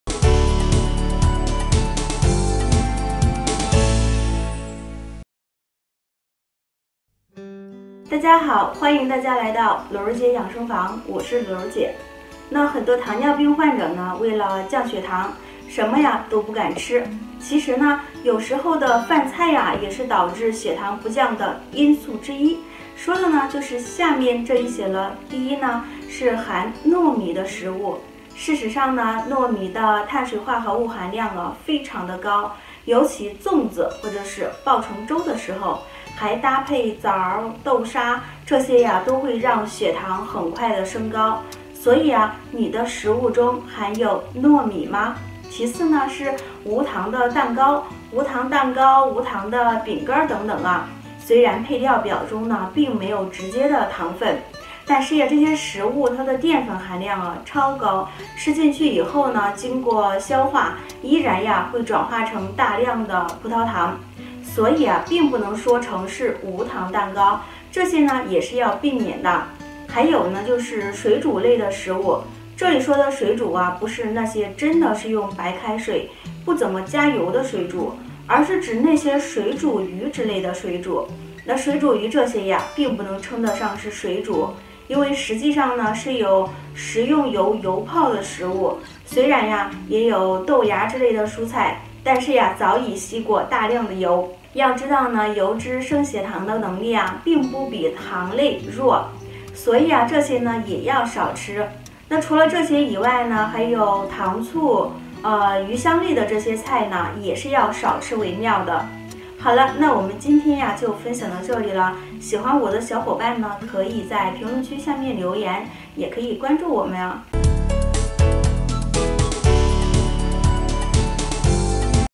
这类食物看似清淡，其实有很多油，多吃只会升血糖！原来想错了
这类食物看似清淡，其实有很多油，多吃只会升血糖！原来想错了,本视频由营养师卤肉姐提供,2次播放,好看视频是由百度团队打造的集内涵和颜值于一身的专业短视频聚合平台